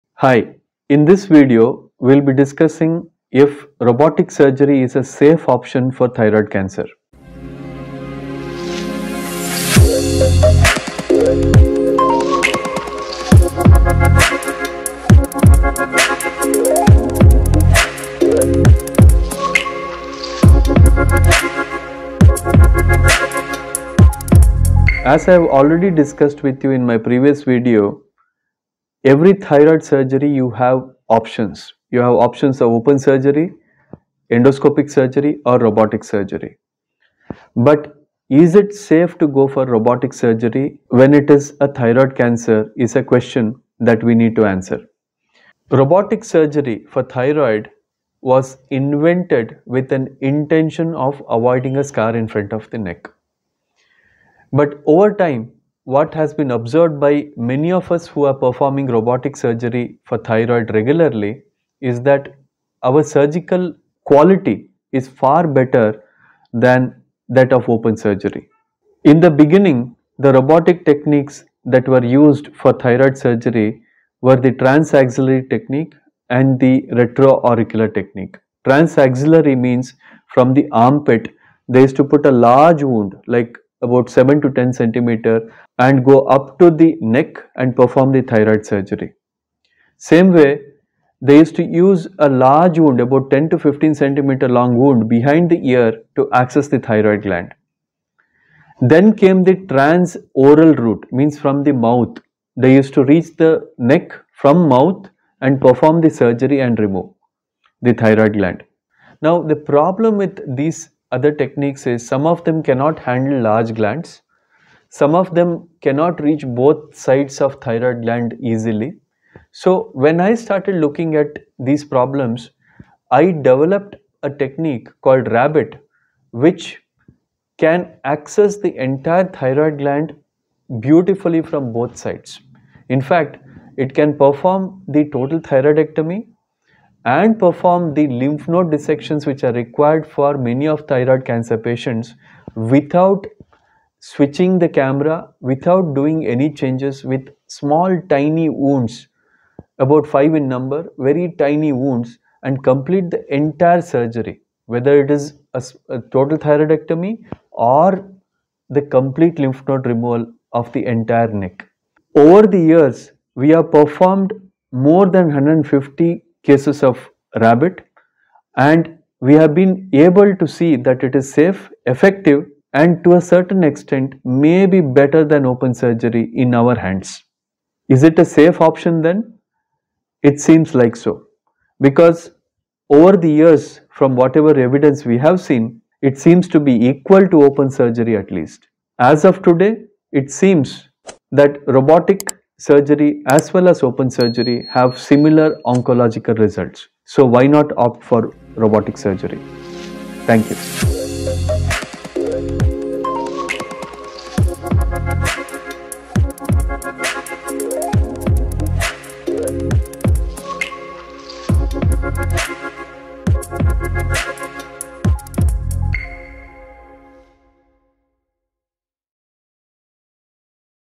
Robotic surgery for thyroid cancer: Safe? | Dr. Sandeep Nayak
In this insightful video, Dr. Sandeep Nayak, a renowned oncologist, delves into the safety and advantages of robotic surgery for thyroid cancer. Learn why robotic surgery is increasingly preferred for treating thyroid cancer and how it compares to traditional surgical methods.

(Online & in-person appointment can be booked online or by call)